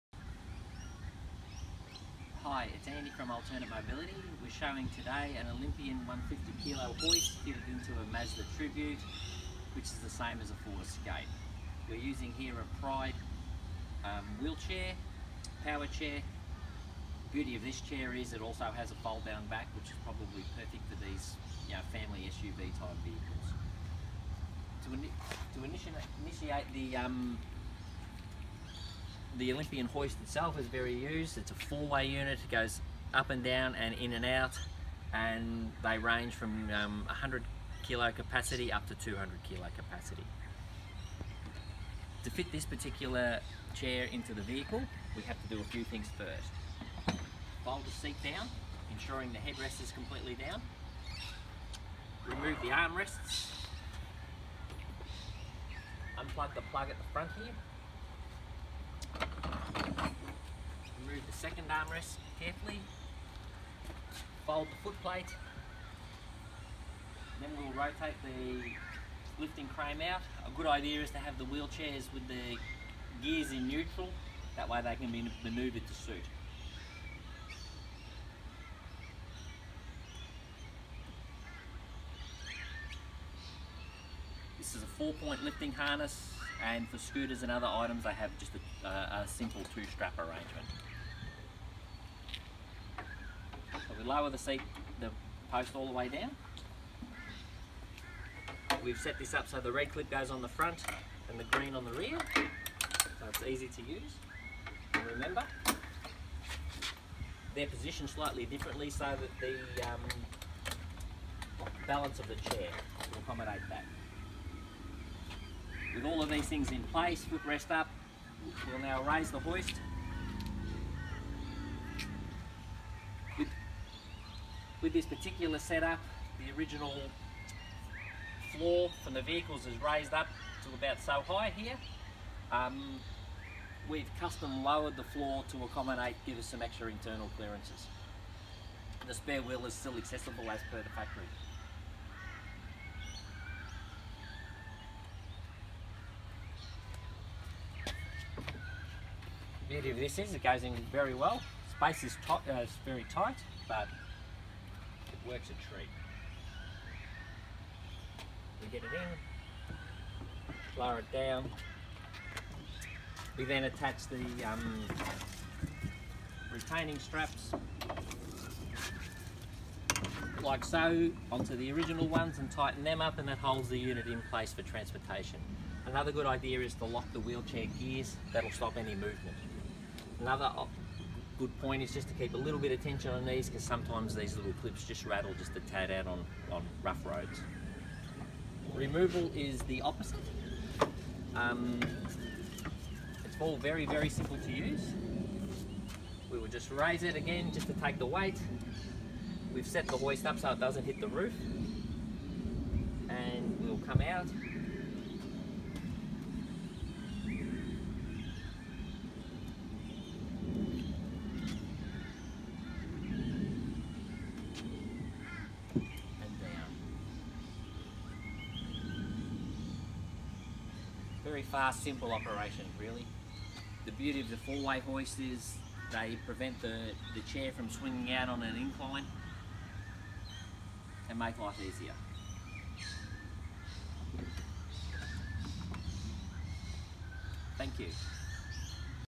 MAZDA TRIBUTE - Vehicle Modification
This installation was complex. The client had been house bound for many years and lives in a remote area.

The wonderful Occupational Therapist had organized a Pride Elite wheel chair. This Wheel Chair is perfect for this application because the back rest fold flat and the armrests are easily removable.

The door opening and general space in side the Mazda Tribute provided a few challenges for this particular chair. We have installed several hoists into these models ( also called a Ford Escape).
Given the weight of the Power Chair we required the 150 KG capacity Auto Chair Olympian hoist. These hoists install up snuggly to one side and the main lifting arm can be removed easily if needed.

The biggest challenge in this instance was balancing the opening height and also having enough slack in the attachments so that the power chair was easily removed from the hoist when unloading and loading.

To provide some additional clearance we removed the original rear floor and custom made a new stronger lower floor.

Not only does the Power chair go in and out easily, but access to the spare wheel is also a simple operation should the need arise.

The main part of installing these hoists is “ease of use” for the end user. We always try a variety of ways and options until we find the most user friendly configuration.